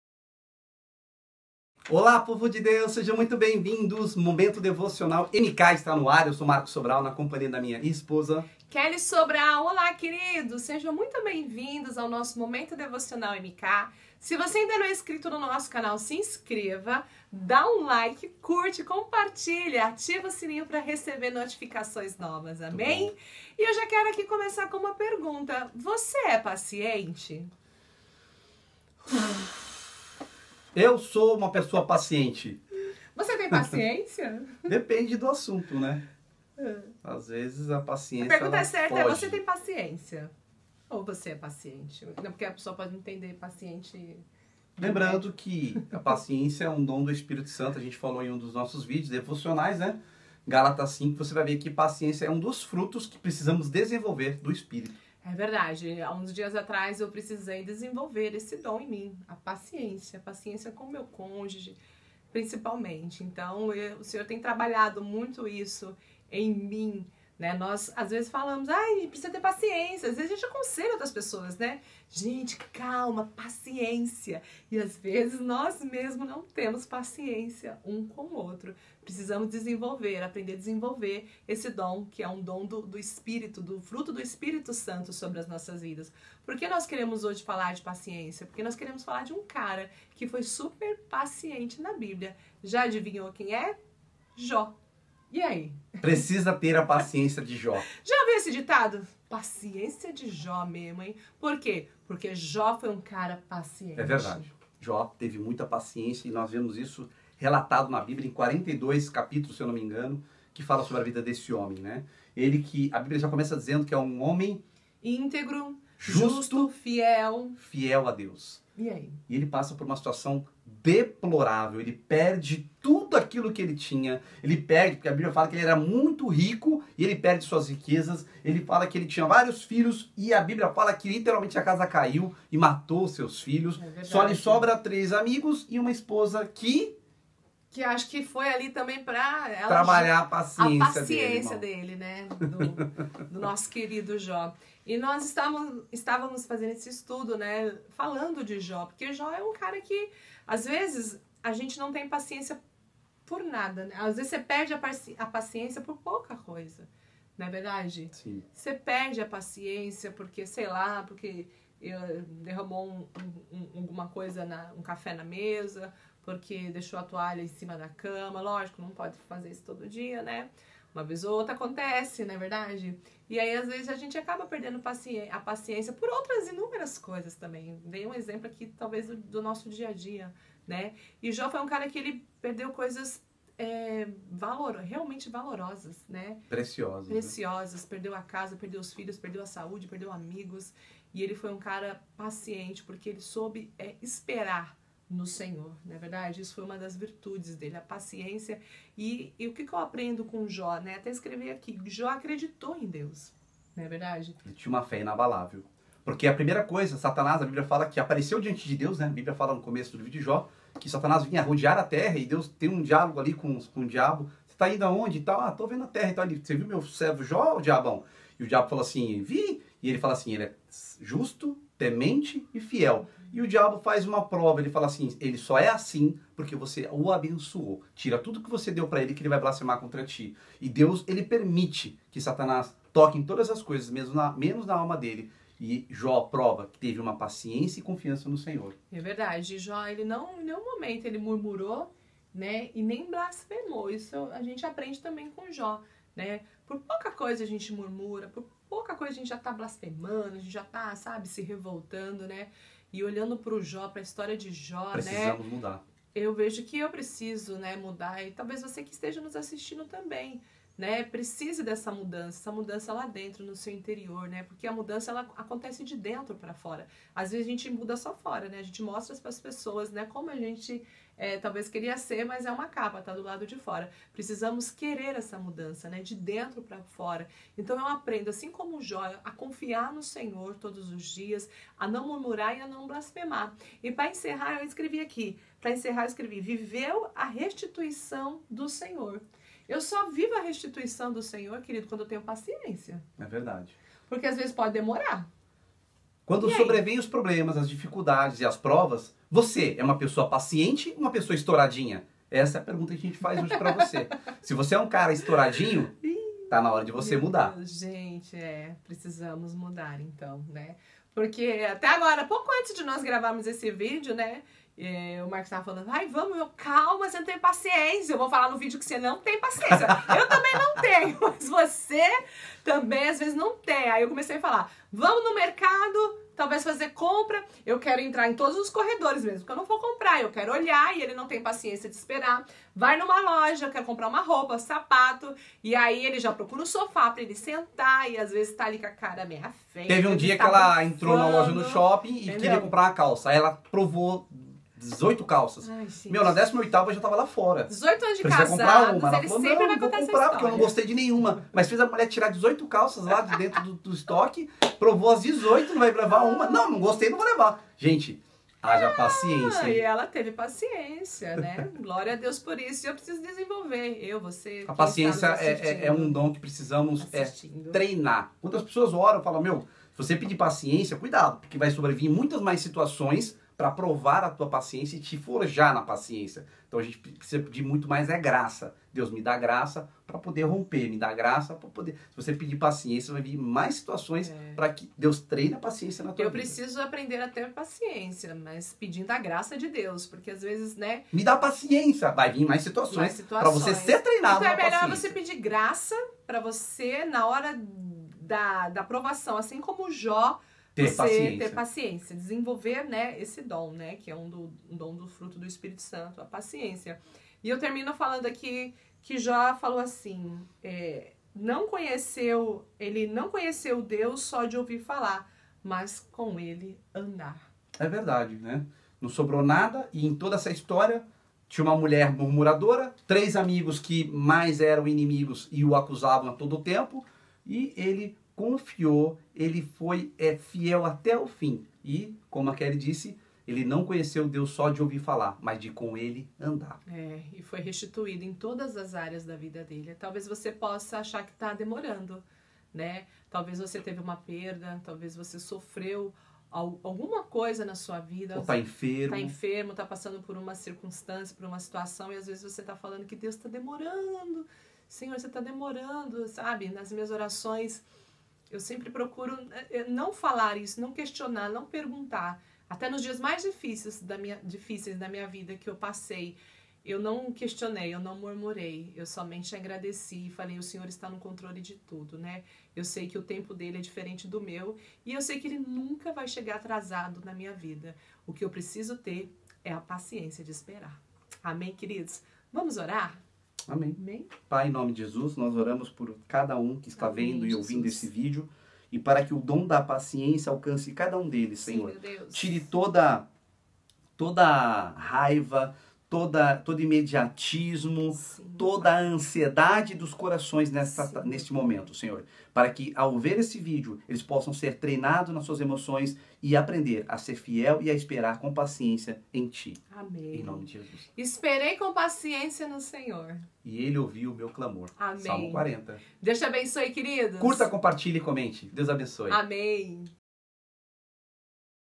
Como exercitar a Paciência ?
Palavra baseada na história de Jó e como ele foi paciente em não murmurar contra Deus, e confiar em sua fidelidade, e como vencer as lutas e batalhas do dia a dia em casal, tendo uma vida a dois de companheirismo, cumplicidade, fidelidade e paciência.